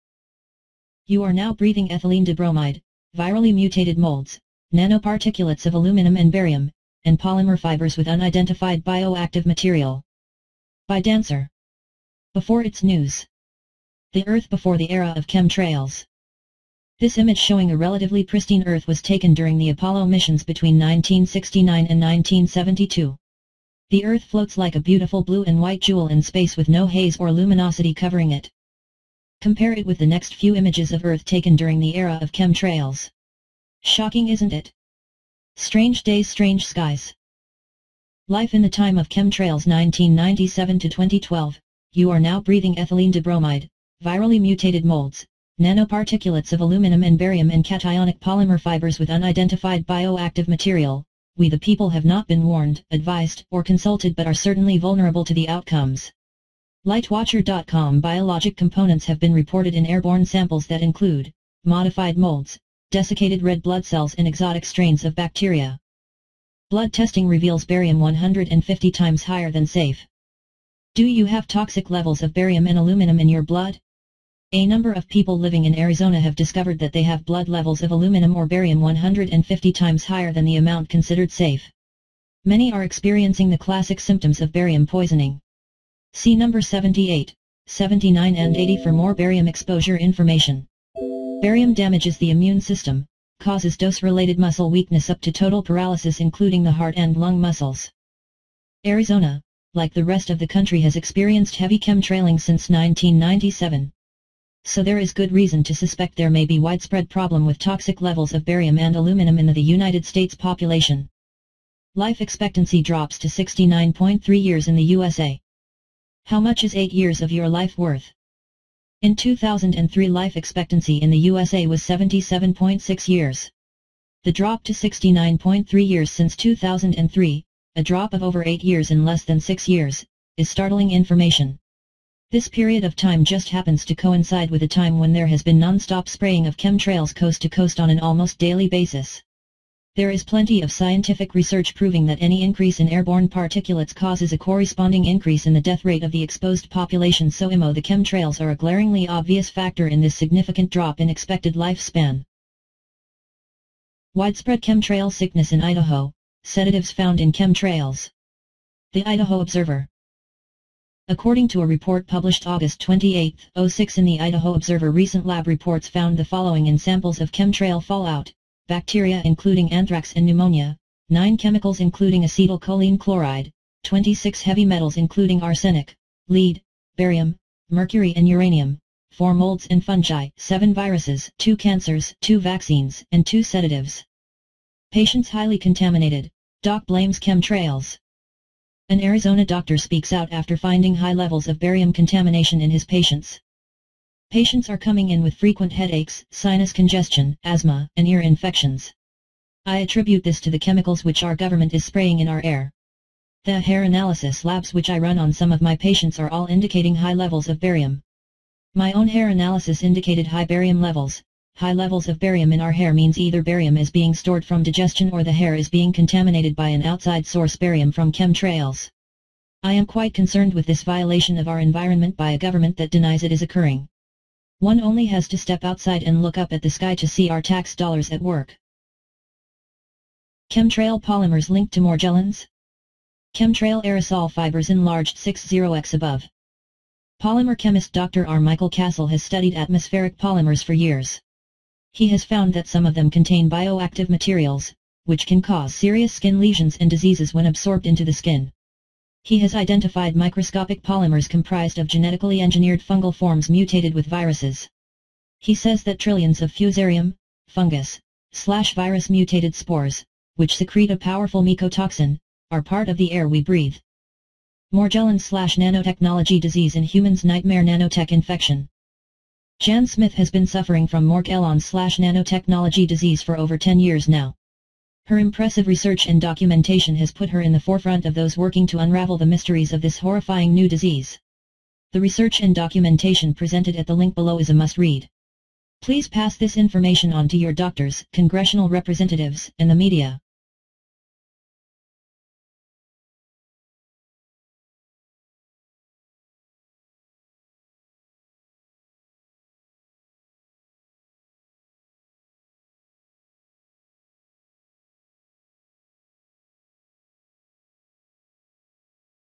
Breathe Virally Mutated Molds, Nano-Particulates, and Polymer Fibers....
You are now Breathing Ethylene Dibromide, Virally Mutated Molds, Nano-Particulates of Aluminum and Barium, and Polymer Fibers with Unidentified Bio-Active Material